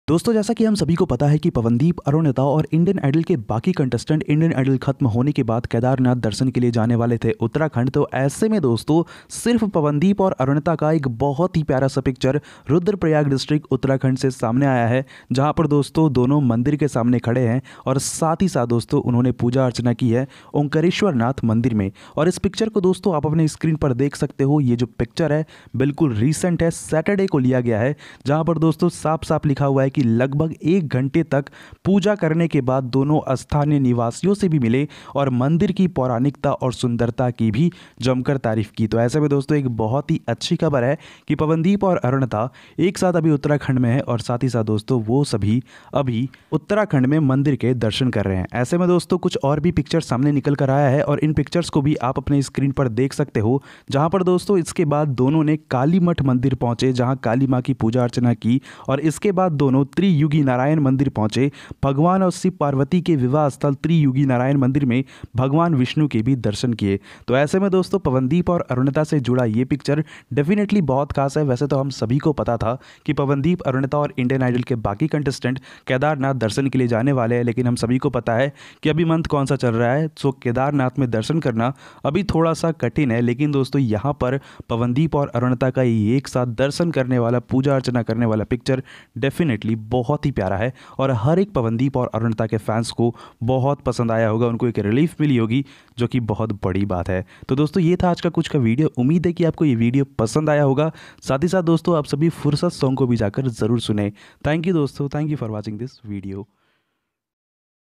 Pawandeep Rajan और Arunita पहुंचे केदारघाटी उत्तराखंड | Arunita In champawat
Guys In this video you find pawandeep rajan and arunita kanjilal reached Uttarakhand for blessings of god and also you get some recent pictures of pawandeep and arunita from Uttarakhand and also get some cute moments of pawandeep and arunita in champawat .

Creator Research